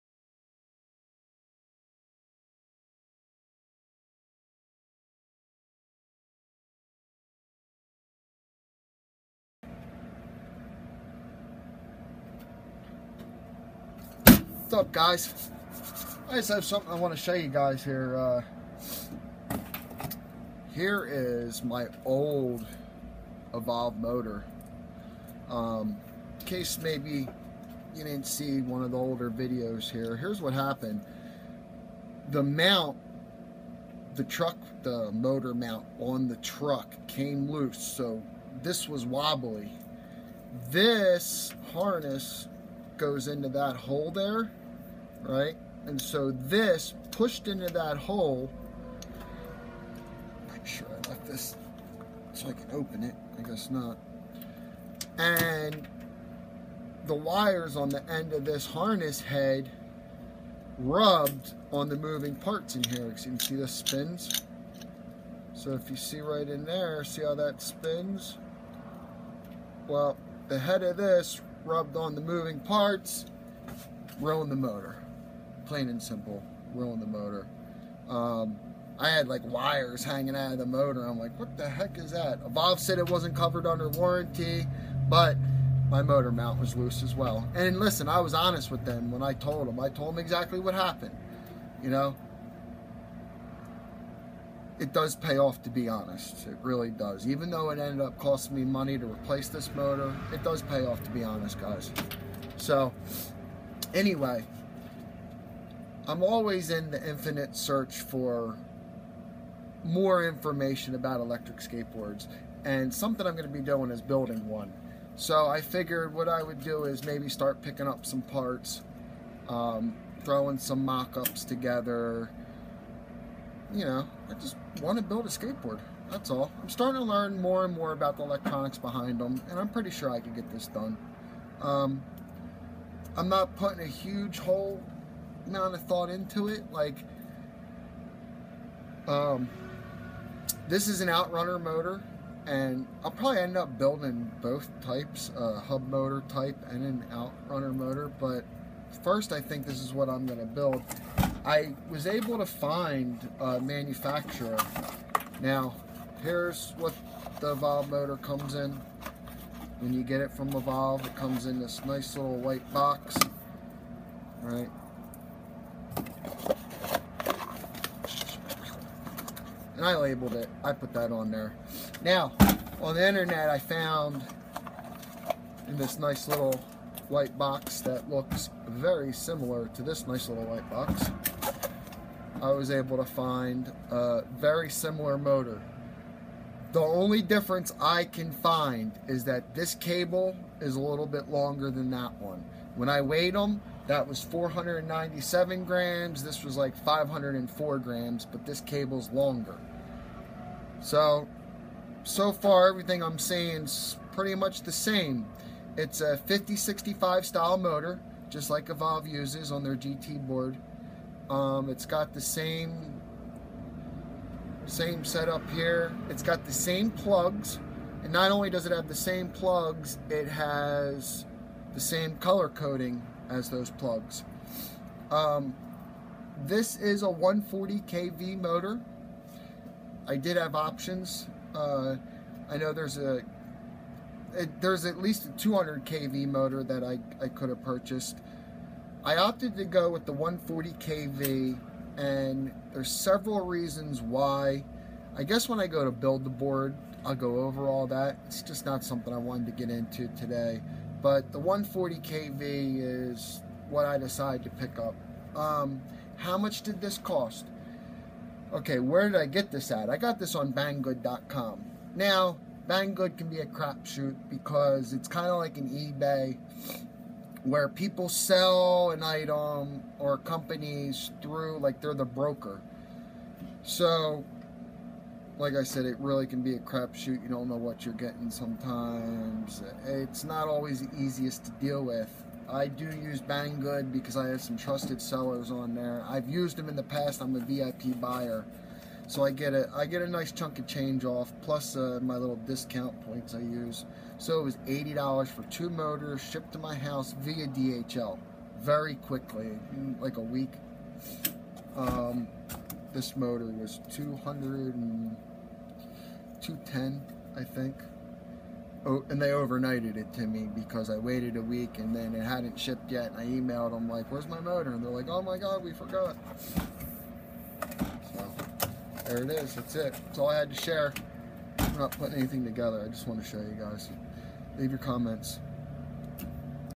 Evolve Gt Motor Issues SOLVED Electric Skateboard Motor Racestar 5065
Heres a link to that motor. It gets good reviews from what I can see other people say about it in the forums.

Evolve Carbon Gt
Evolve GT Electric Skateboard